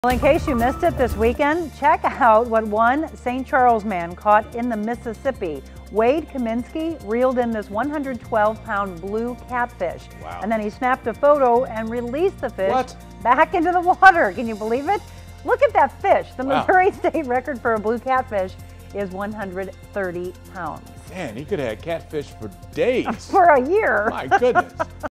'Fish of a lifetime' | St. Charles angler catches 112-pound catfish on Mississippi River
ST. LOUIS — A St. Charles man has one heck of a big fish story to tell after a day out on the Mississippi River.

Wade Kaminski didn't just land any catfish on Friday. He reeled in a 112-pound, approximately 5-foot-long monster Blue Catfish.

“I initially thought I got into a snag," Kaminski said. "I told my friend who was with me, 'I’m either snagged or I’ve got a monster'.”

It turned out to be the monster.

The fish put up an eight-minute fight before Kaminski was able to get her into the boat, where fist bumps and high fives followed.

"It was the biggest and baddest fight I've ever been involved with," Kaminski said.

Once the fish was in the boat, Kaminski even needed a different scale brought in to properly weight the fish, since the one he had in his boat only went up to 100 pounds.

"This was the one I’ve been chasing my whole life," Kaminski said.

The angler said the Blue Catfish appeared to be female and had already spawned. So it likely weighed even more a few weeks ago.

The Missouri state record for a Blue Catfish is 130 pounds.

Kaminski released his prize catch back into the Mississippi River, so future fishermen will have the chance to experience the thrill he did.

“We want these fish to be around for the next generations or maybe in a couple years to catch this fish again and get the same enjoyment we got today," Kaminski said.